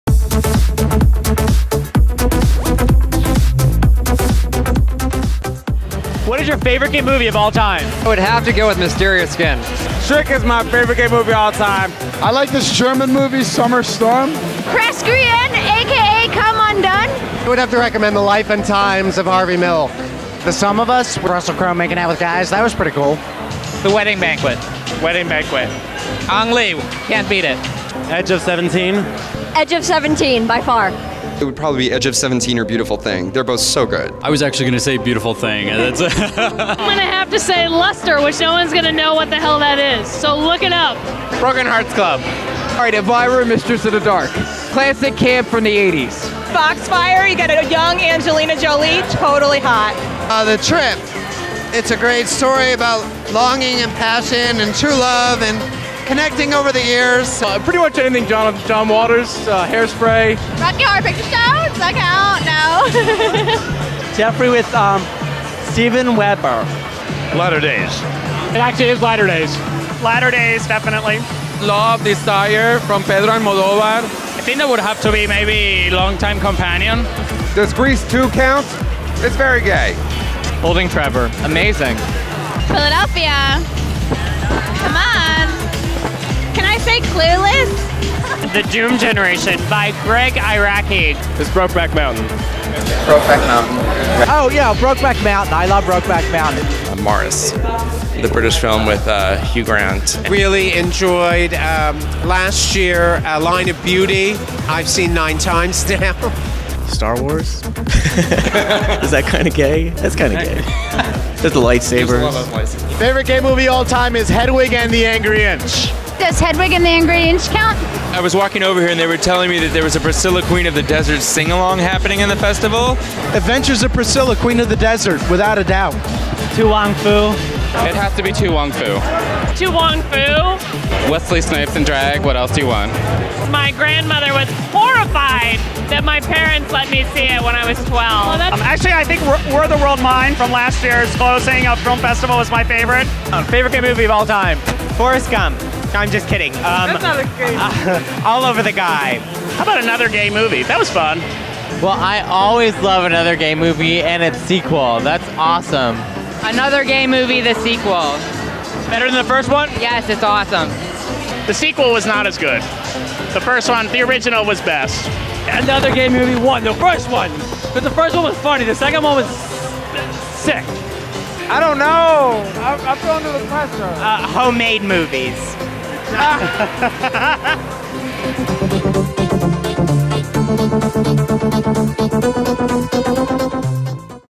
QFest 2009: Favorite Gay Movie?
PhillyGayCalendar.com asks QFest 2009 attendees, "What's your favorite gay movie of all time?"